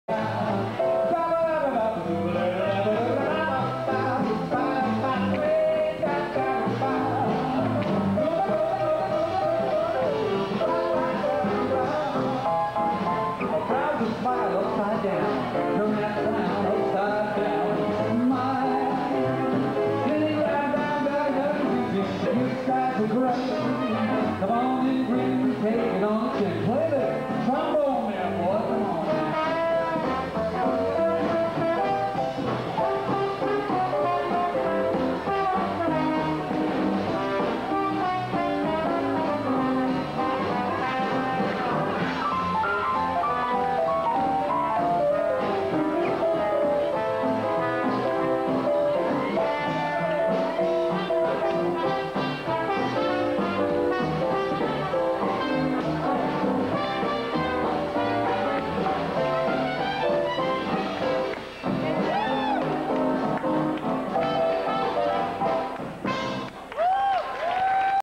1999 beantown lindy camp July 2 night time dances sweedish dance teachers dancing
1999 beantown lindy camp July 2 night time dances sweedish dance instructors dancing